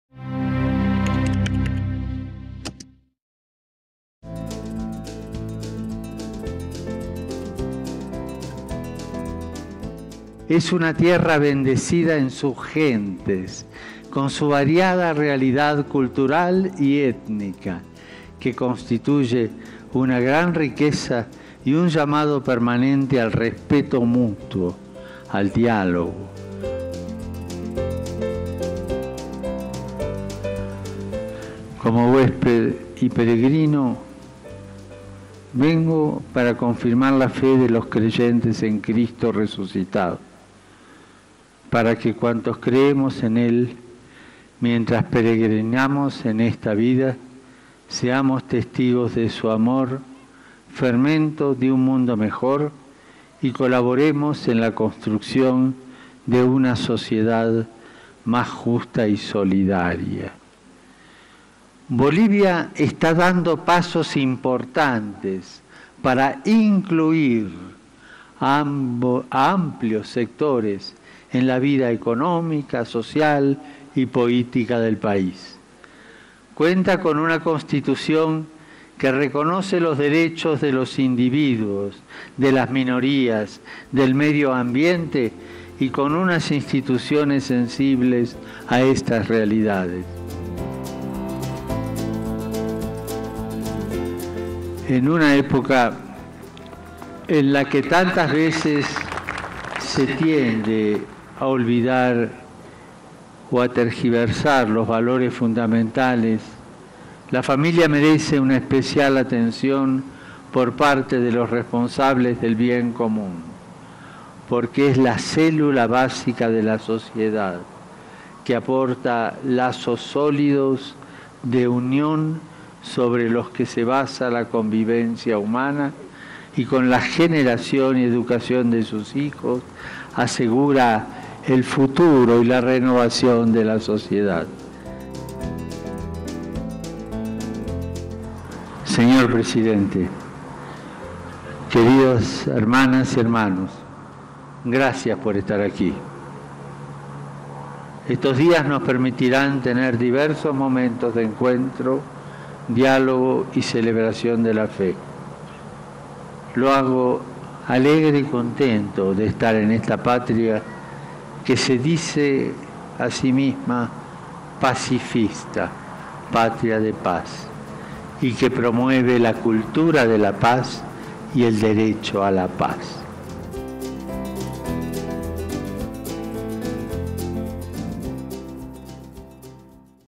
Pope Francis arrives in Bolivia
Pope Francis spoke about the importance of the family during his address to the President of Bolivia July 8.